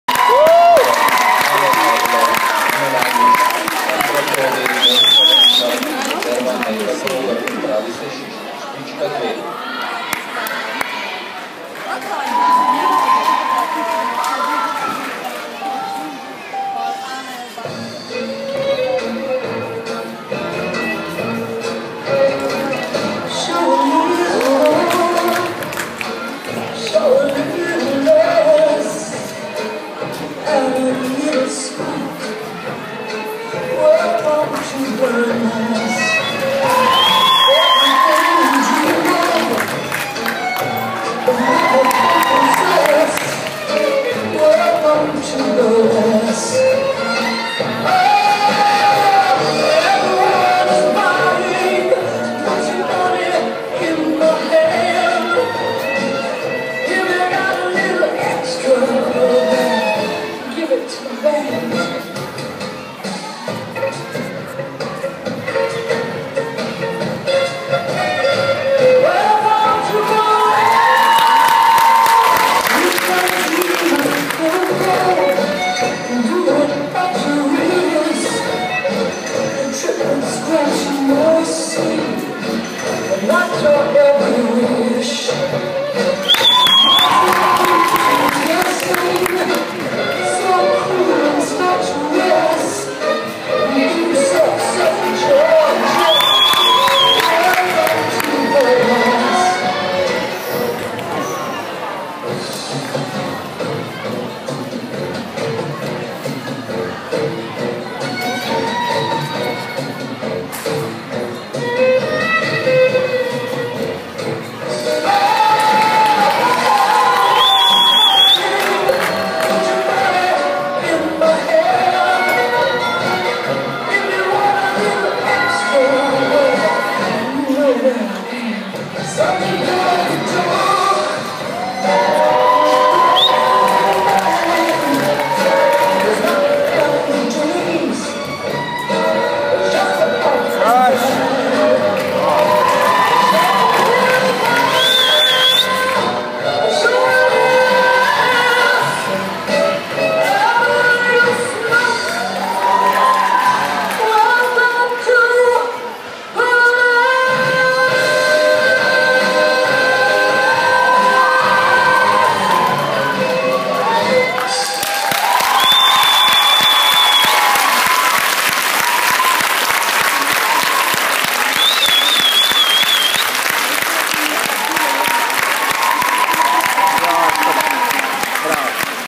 Rhythmic Gymnastics - 7 Balls - SOKOL BEŽIGRAD ZALA 3
Članska ekipa Športnega društva Sokol Bežigrad ZALA 3 na 2. prvenstveni tekmi ritmične gimnastike programa A1 na Vrhniki.
S skupinsko vajo z žogami so se uvrstile na 3.mesto.

Slovenija, 2016